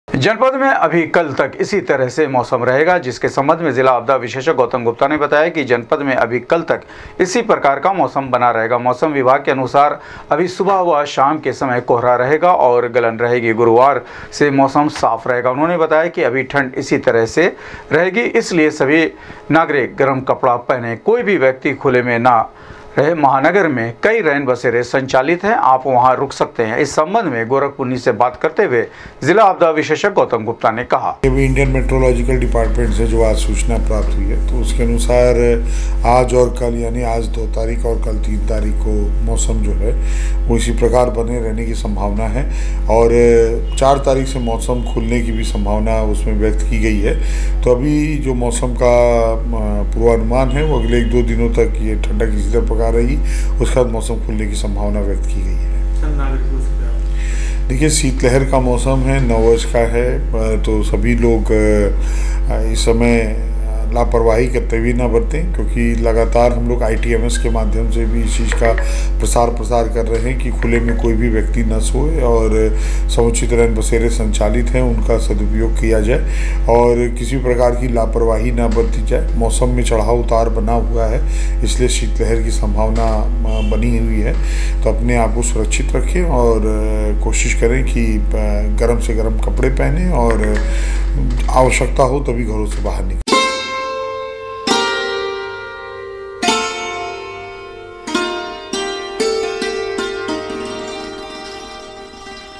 जनपद में अभी कल तक इसी तरह से रहेगा मौसम
जनपद में अभी कल तक इसी तरह से रहेगा मौसम,सुबह व शाम में कोहरे के साथ रहेगी ठंड,गुरुवार से मौसम हो सकता है साफ/&